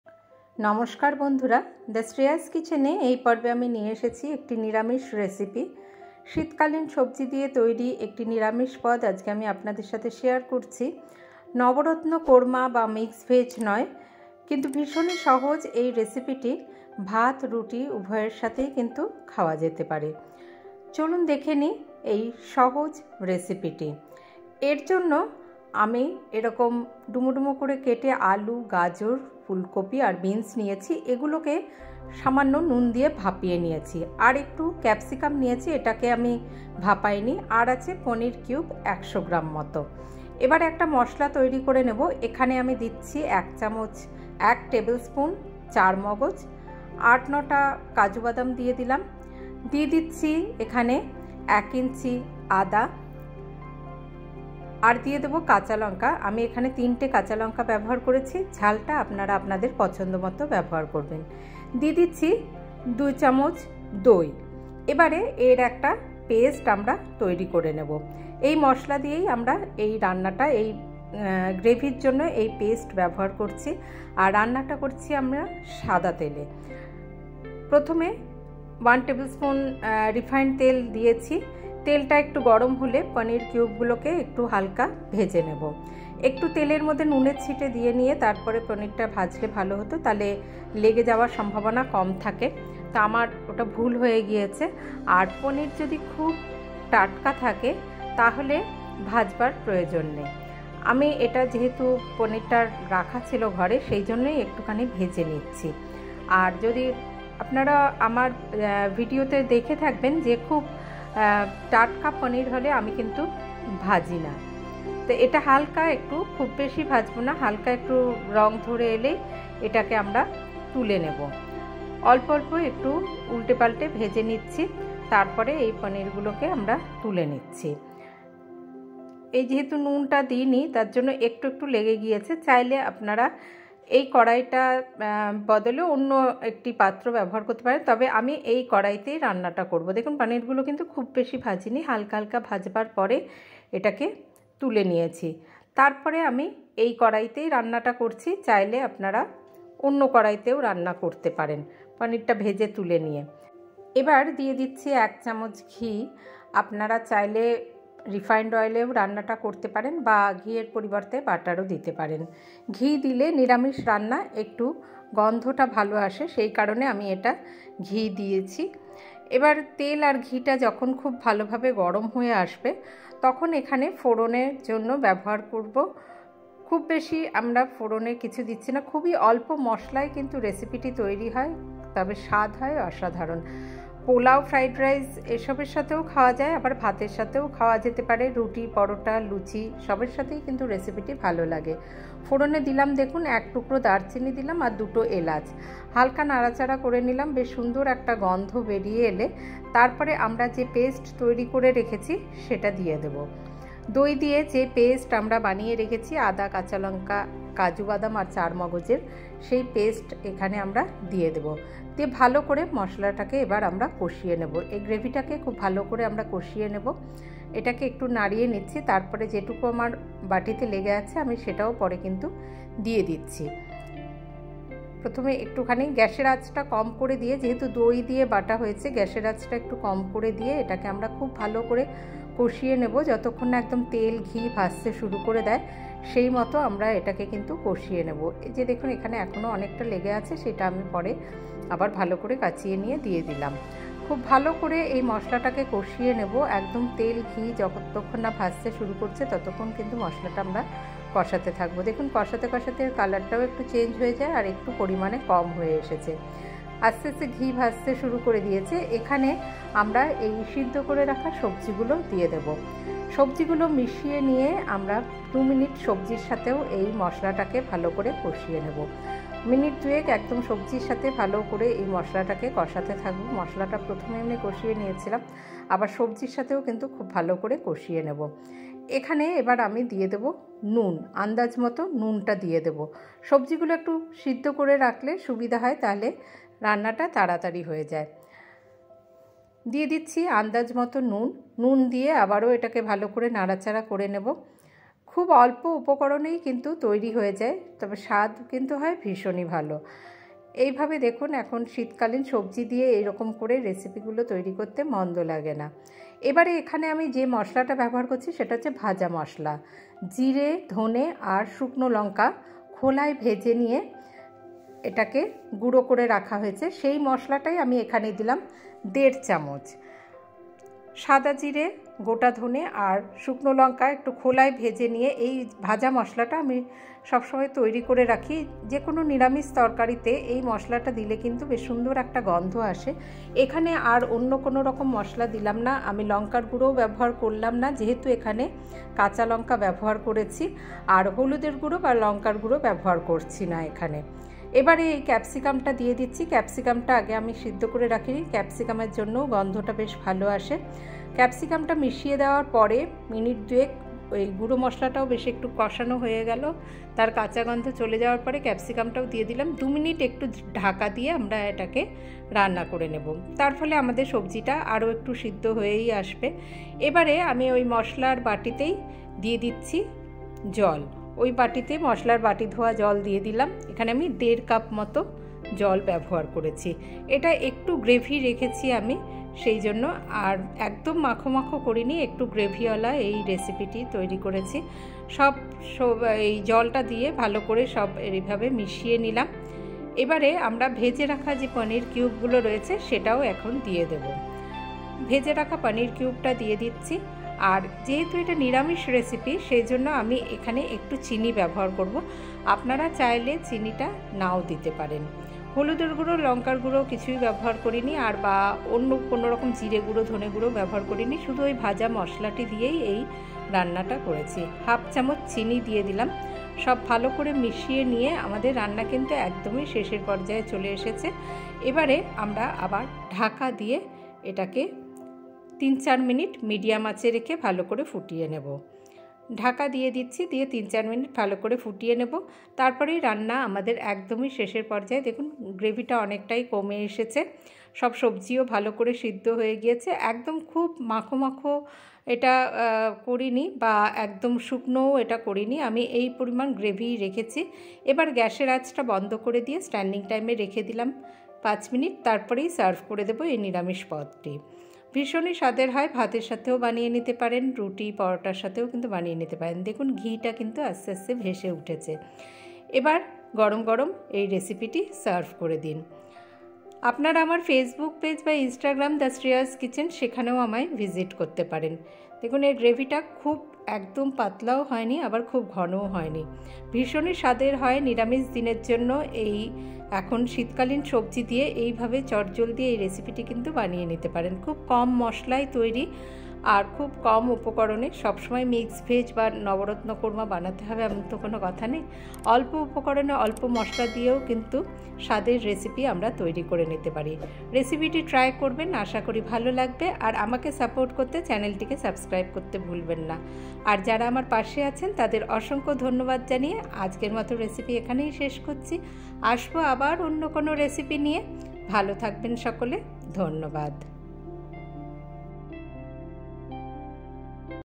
নিরামিষ দিনে কম মসলায় তৈরি এই পদ ভাত রুটি উভয়ের সাথেই দারুণ জমে।vegetarianrecipe
Recipe by Shrabani Sen।
cooking recipe
cooking video
vegetarian recipe
easy recipe
tasty recipe
mixed veg recipe
lunch recipe
dinner recipe